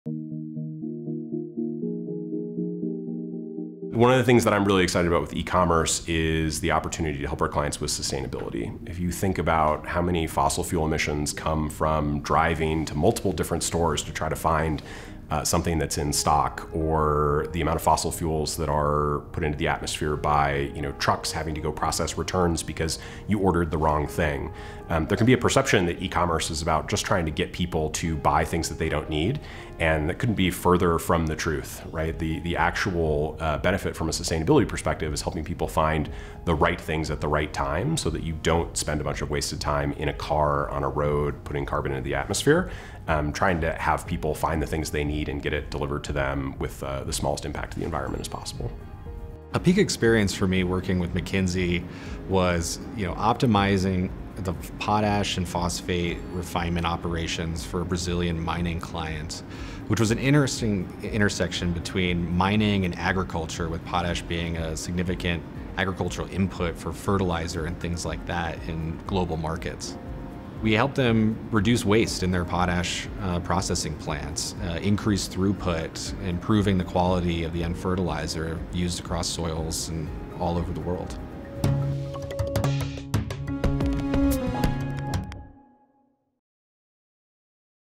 McKinsey Digital: Sustainability and the Role of Tech
Meet Jake, an expert associate partner, and Jon, a principal software engineer, as they takes us through how their projects with e-commerce, mining and agriculture allowed them to address sustainable solutions to more efficiently tackle environmental issues.